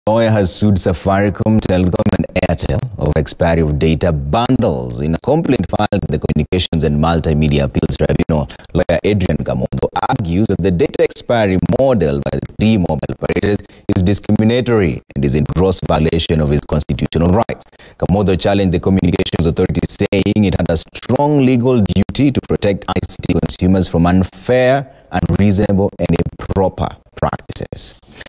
Lawyer sues mobile operators over unused data bundles
A lawyer has sued Safaricom, Telkom and Airtel over expiry of data bundles.
In a complaint filed at the Communications and MultiMedia Appeals tribunal, lawyer Adrian Kamotho argues that the data expiry model by the three mobile operators is discriminatory and is in gross violation of his constitutional rights. Kamotho challenged the Communications Authority saying it had a strong legal duty to protect ICT consumers from unfair, unreasonable and improper practices.